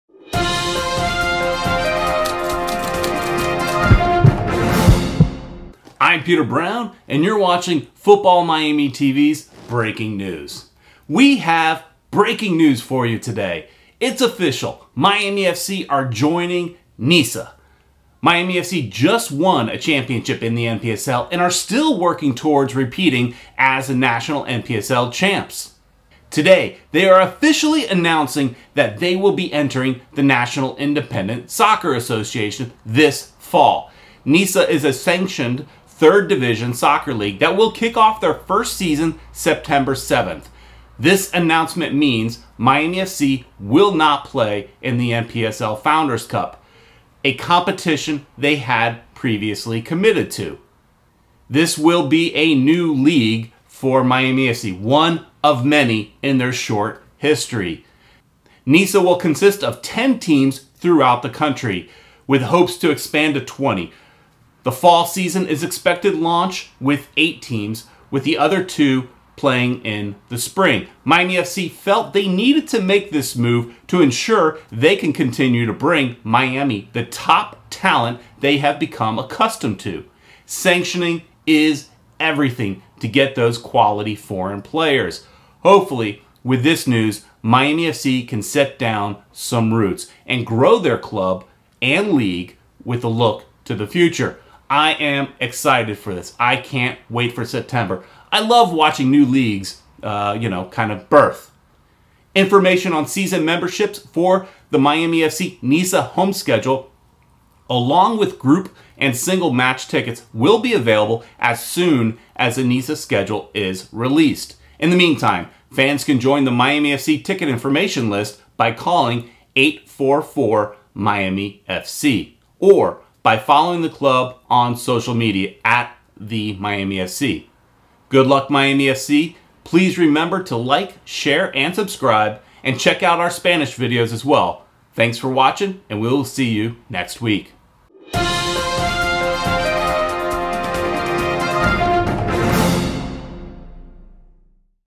BREAKING NEWS: Miami FC Join NISA | Local News | 7/24/19
BREAKING NEWS: Miami FC will join the National Independent Soccer Association this Fall. This means they will not be playing alongside Miami United in the NPSL Founders Cup. NISA will consist of 10 teams launching with 8.